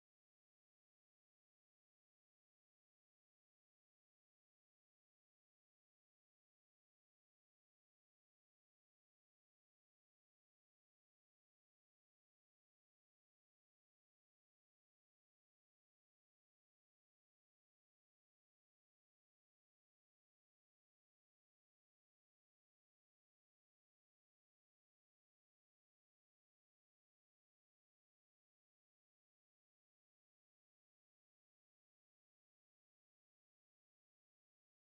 The Fry Station In Motion
A video that shows how the fry station works!